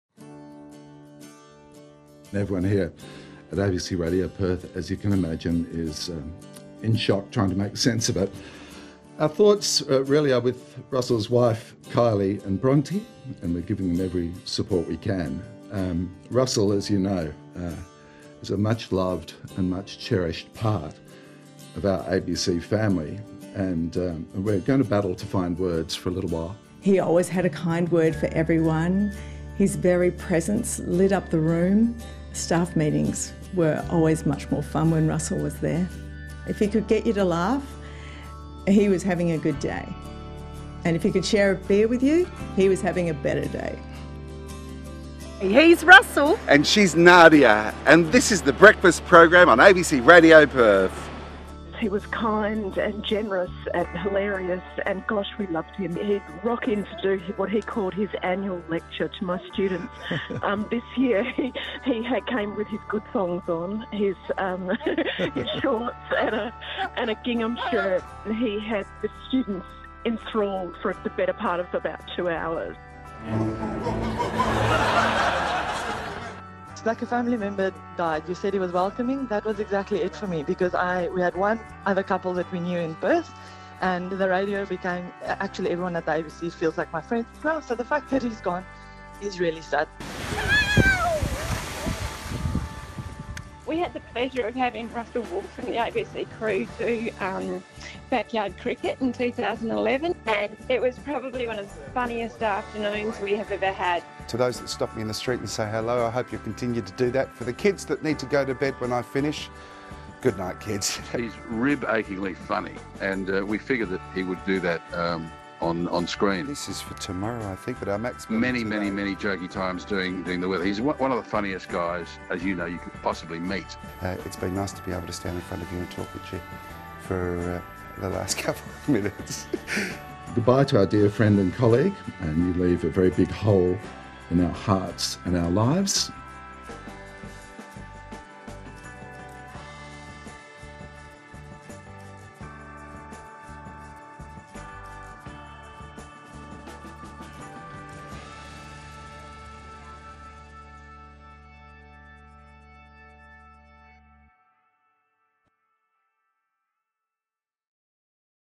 Tributes are flowing in for ABC presenter Russell Woolf. | ABC news
Radio presenter Russell Woolf was a man with a big heart and a host of friends.

From radio to TV, to on stage with the Queen, Woolf brought an easy going charm and a larrikan humour to everything he did.